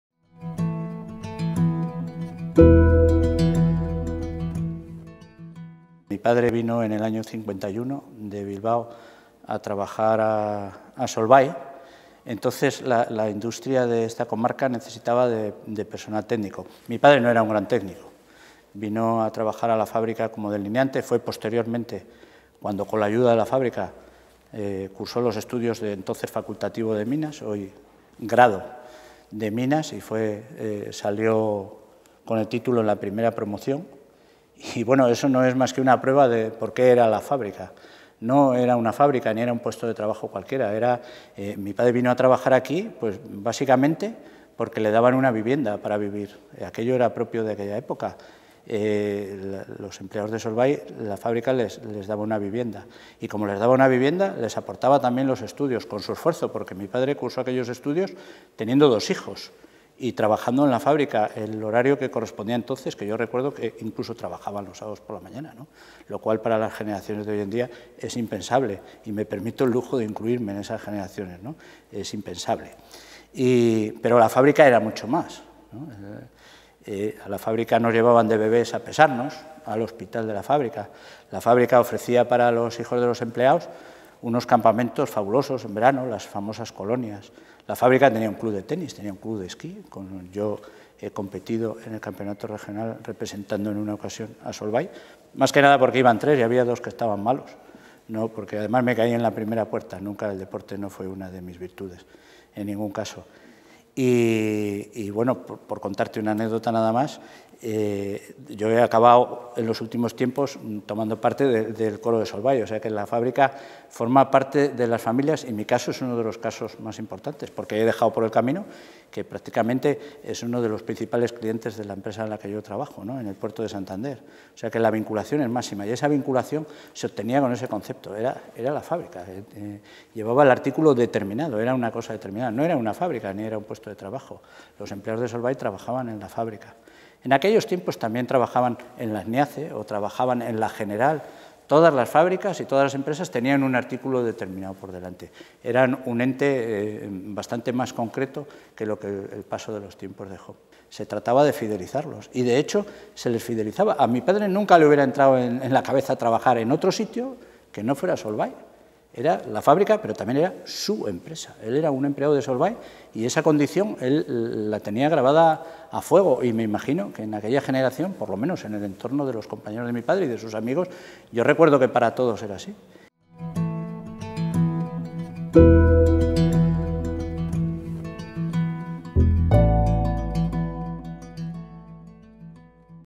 Solvay - Trabajadores a mediados del siglo XX
Solvay forma parte de la vida industrial de Torrelavega y el Besaya desde hace más de un siglo. Victor Conde Dueñas, hijo de un antiguo trabajador de Solvay, nos cuenta sus recuerdos sobre la fábrica.
En Primera Persona es un proyecto de recuperación del patrimonio inmaterial de Cantabria a través de la tradición oral.

00´00´´ -  02´30´´ Hijo de trabajador de Solvay. Su padre vino aquí porque le formaron y le daban trabajo y vivienda.
02´21´´ - 02´52´´ . Ofrecia además instalaciones deportivas, campamentos, hospital..